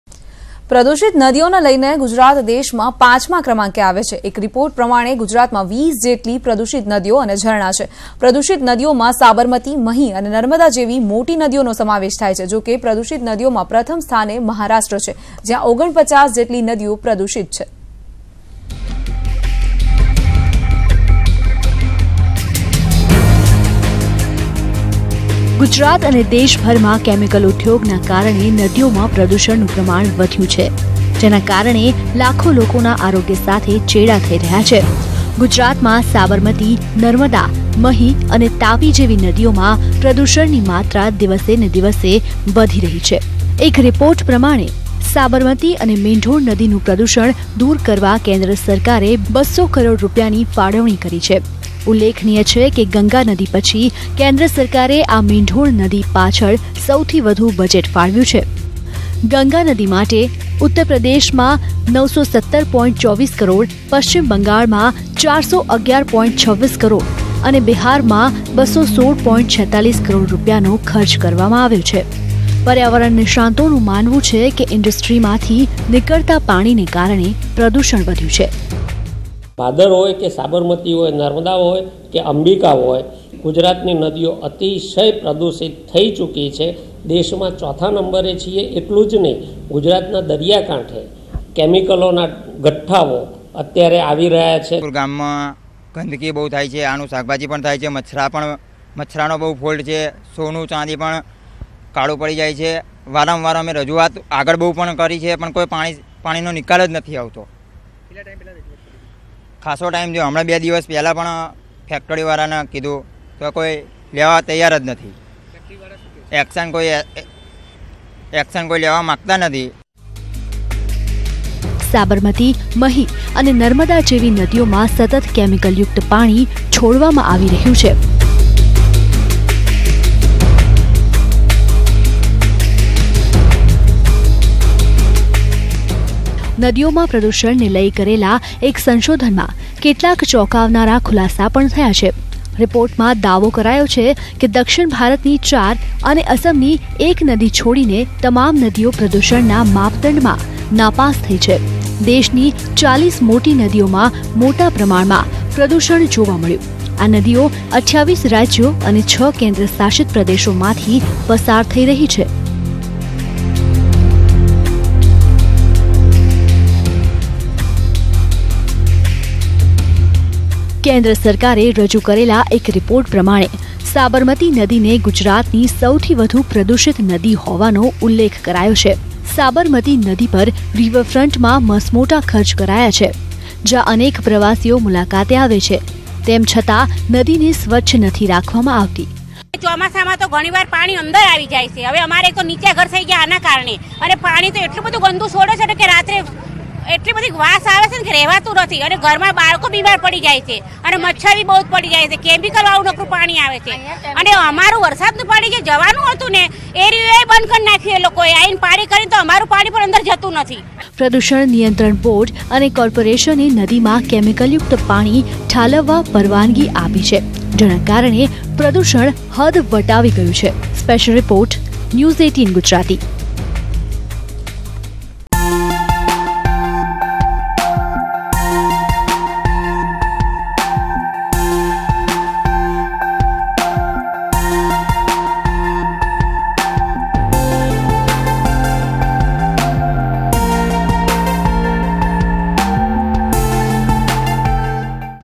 Gujarat among Top 5 in Most Polluted Rivers in India | News18 Gujarati
Gujarat among Top 5 in Most Polluted Rivers in India.